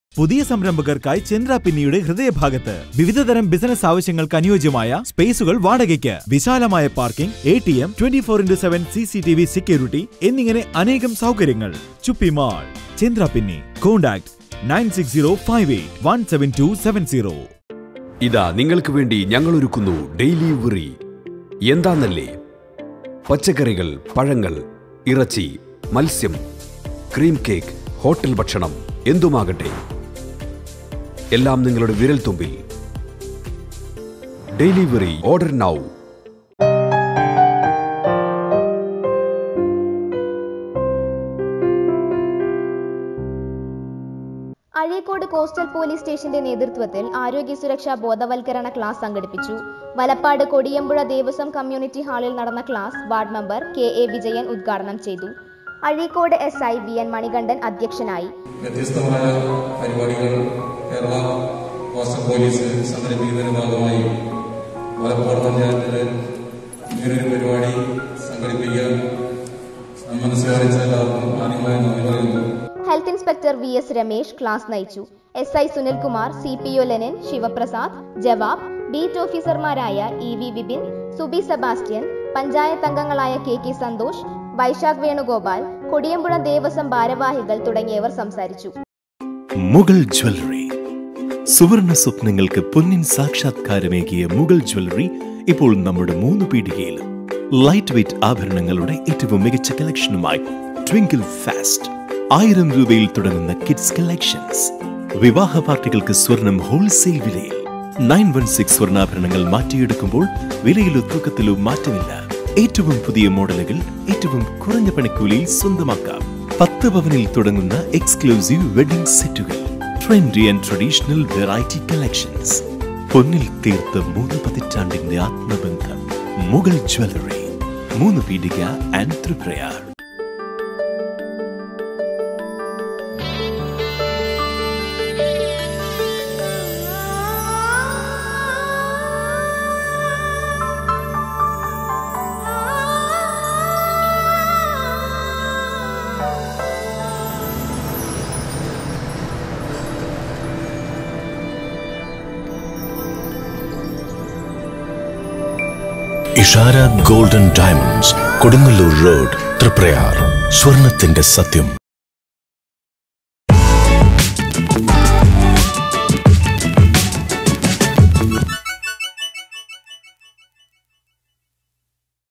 അഴീക്കോട് കോസ്റ്റൽ പോലീസ് സ്റ്റേഷൻ്റെ നേതൃത്വത്തിൽ ആരോഗ്യ സുരക്ഷ ബോധവൽക്കരണ ക്ലാസ് സംഘടിപ്പിച്ചു
*dailyvery*
Available pin code:- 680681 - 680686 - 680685